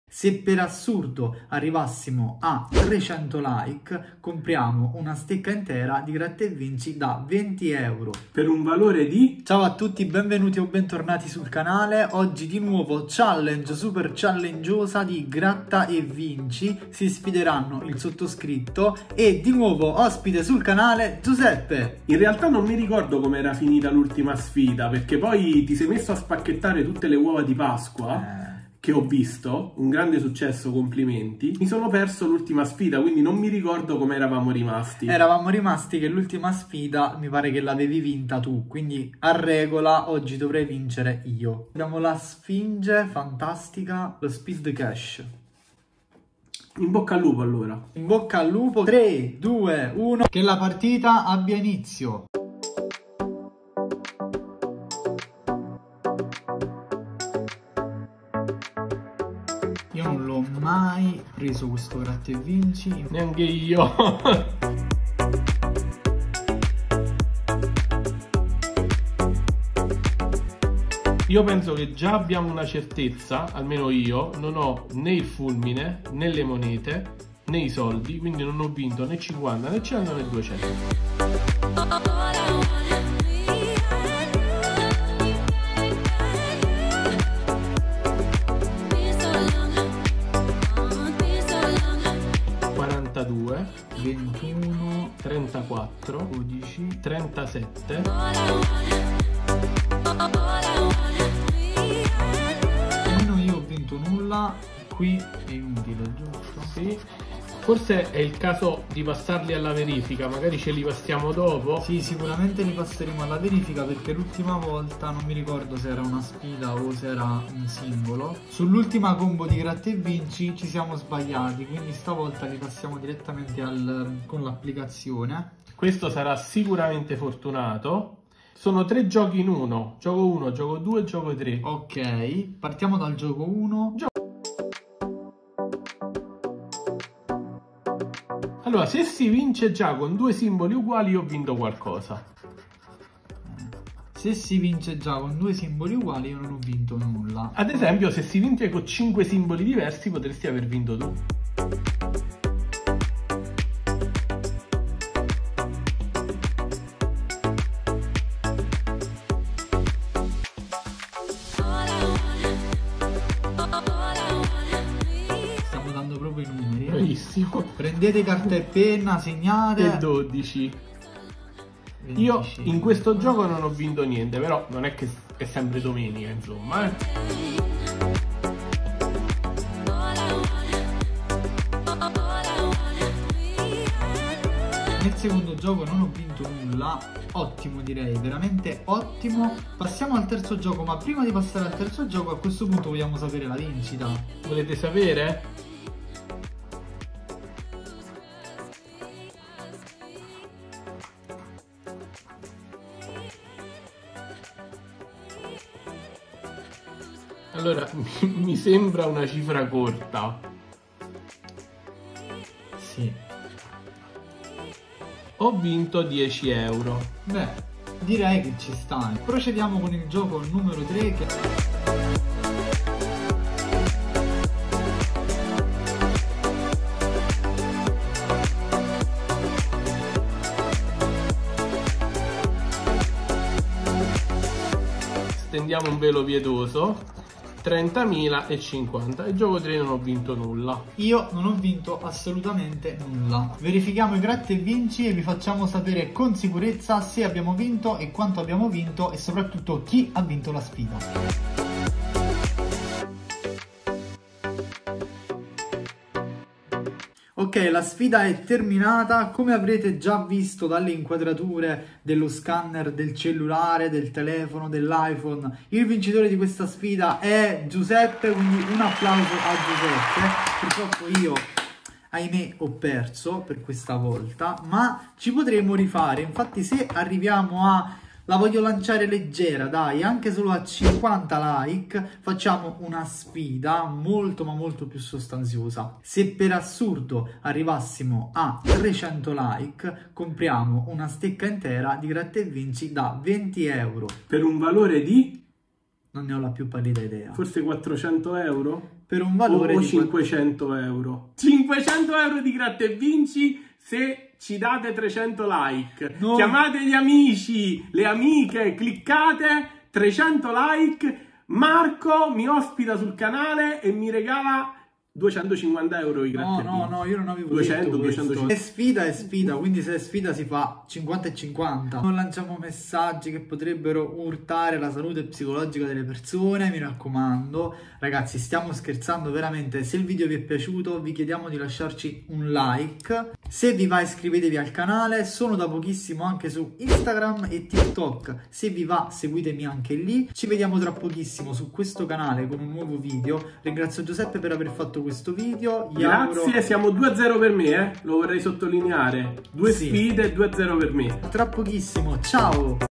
SFIDA GRATTA E VINCI 300 LIKE compro stecca MAXI MILIARDARIO
CHI TIFATE MARCO O GIUSEPPE? Rieccoci tornati con un altra super sfida!

ME CONTRO TE 2022 Apro uova di pasqua ME CONTRO TE con SUPER SORPRESA 250 grammi